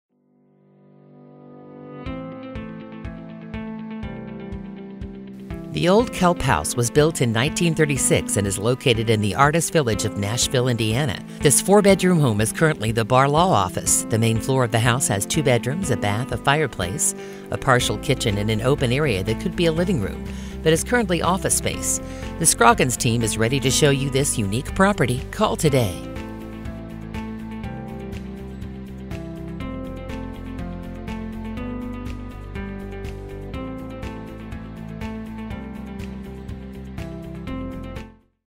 12 S Jefferson Street Nashville, IN | MLS# 21725678
12 S Jefferson Street, Nashville, IN

The "Old Kelp House" was built in 1936 and is located in the artist village of Nashville Indiana. This 4 bedroom house is currently The Barr Law Office. The house is zoned RB allowing for the possibility of several uses: Tourist Rental, office space, or apartments. Main floor of the house has 2 bedroo